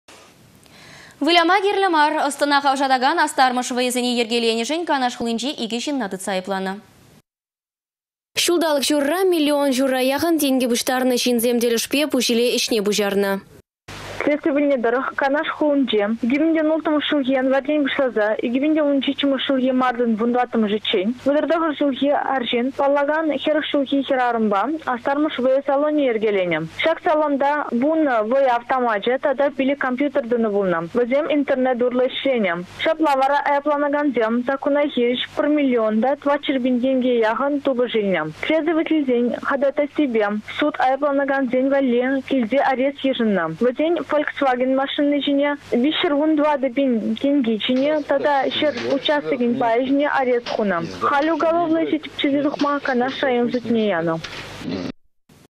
Выляма кирлĕ мар
Ăс-тăна хавшатакан астармăш вăйăсене йĕркеленешĕн Канаш хулинче 2 çынна тытса айăпланă.